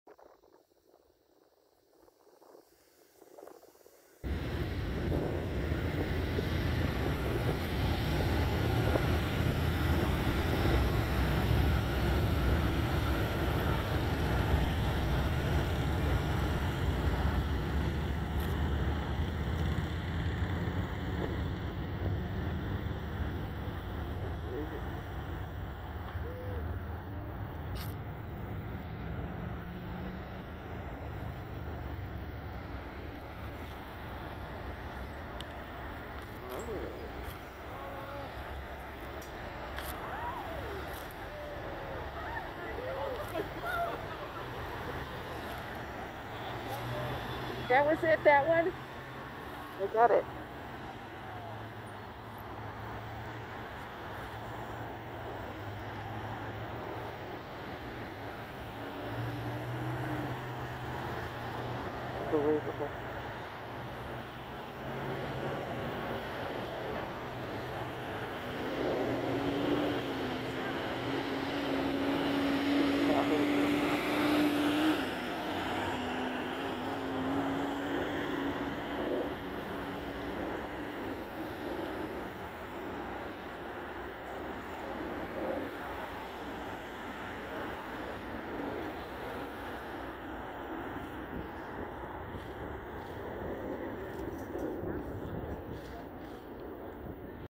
President Joe Biden's Motorcade Part 2, going past Washington Monument to the White House, 1/20/21
President Joe Biden's Motorcade Part Two, going past Washington Monument to the White House, 1/20/21